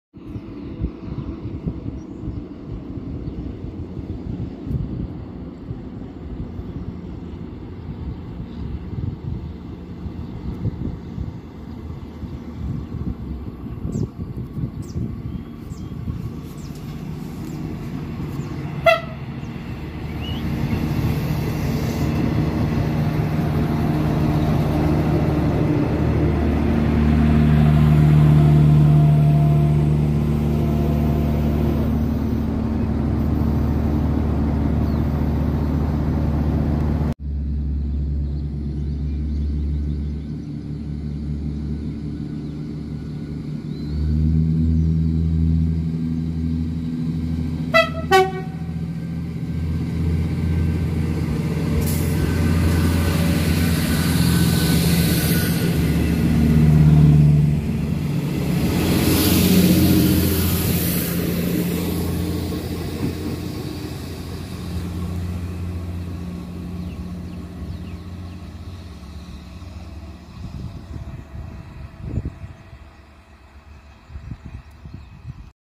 MD 599 a Badajoz circulando por Guareña
Tren 599.043 como Media Distancia 35352 procedente de Castuera y destino Badajoz entra y sale de vía 1.
Grabado el 23 de marzo de 2024 a las 13:55.